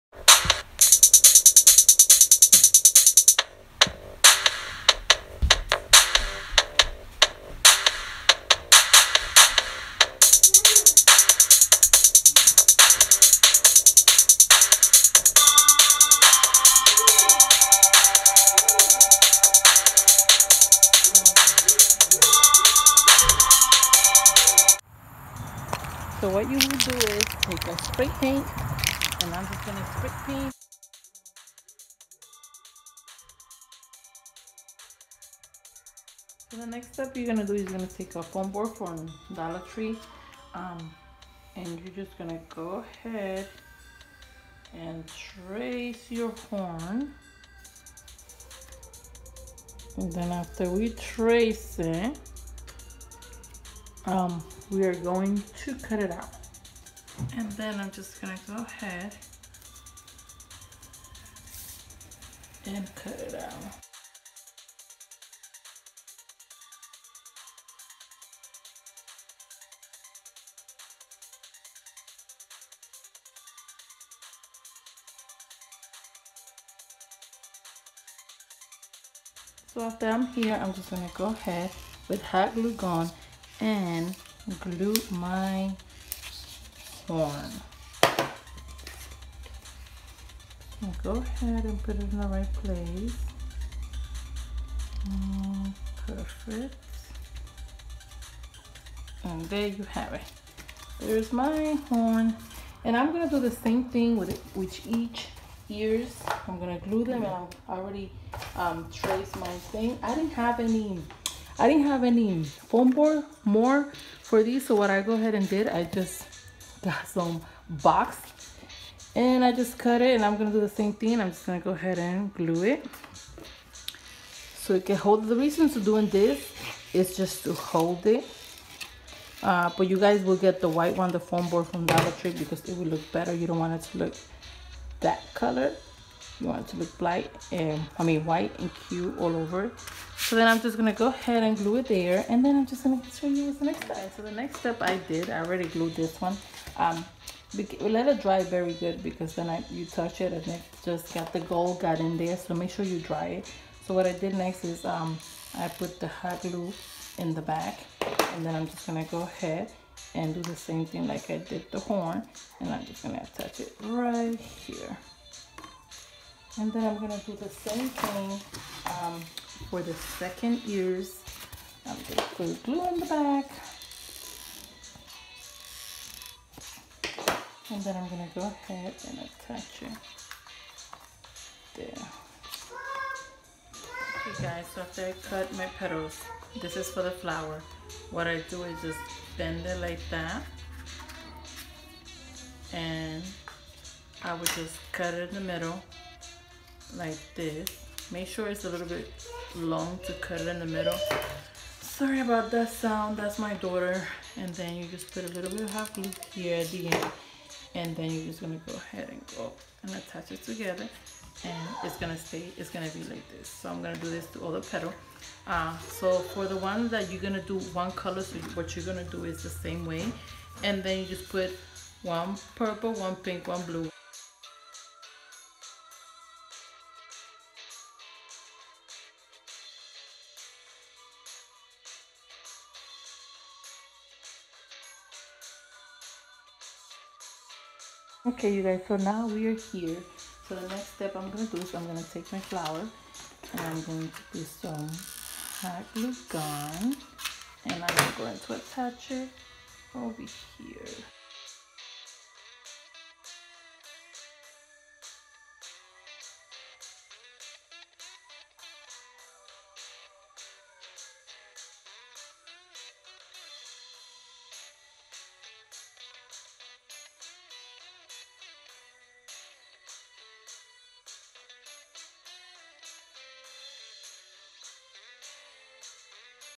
UNICORN HULA HOOP PHOTOBOOTH FRAME/BACKDROP
THIS IS A BEAUTIFUL UNICORN PHOTO BOOTH FRAME OR BACKDROP YOU CAN USE IT HOW EVER YOU WOULD LIKE.
SIMPLE TO MAKE WITH A HULA HOOP FROM DOLLAR TREE.